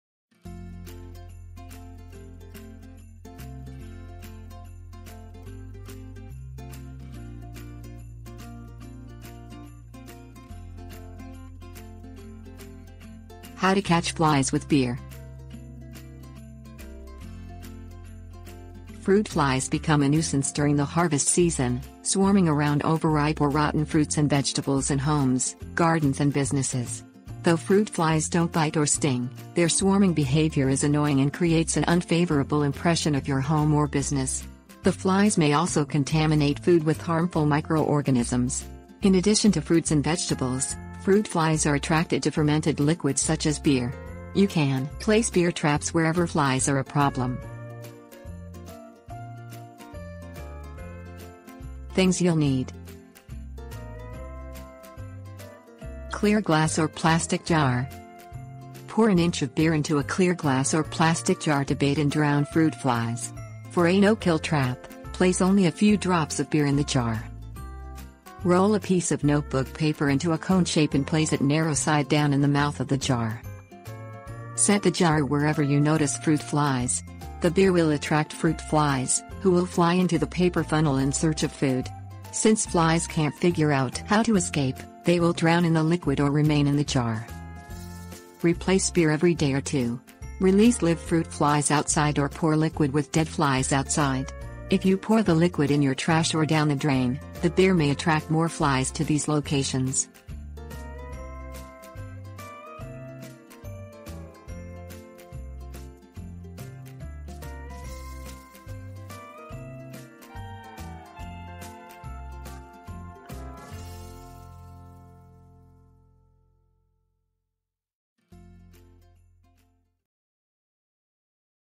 How to Catch Flies With Beer
How to Catch Flies With Beer. Fruit flies become a nuisance during the harvest season, swarming around overripe or rotten fruits and vegetables in homes, gardens and businesses. Though fruit flies don't bite or sting, their swarming behavior is annoying and creates an unfavorable impression of your home or business. The flies may also contaminate...

Table of contents How to Catch Flies With Beer
Things You'll Need 00:50